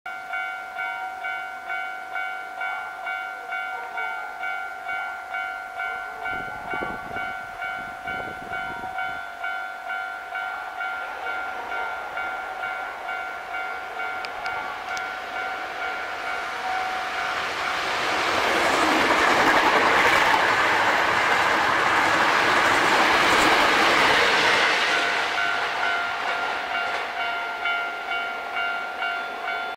2022.10.09 阪急 9300系9309F 快速急行大阪梅田行き通過 摂津市駅～正雀駅間 正音寺踏切 阪急電鉄 阪急電車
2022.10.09 摂津市駅～正雀駅間 正音寺踏切
9309F 8両編成
(Mc1)9309-(T1)9859-(T2)9879-(T2)9889-(T2)9899-(T1)9869-(M1)9809-(Mc2)9409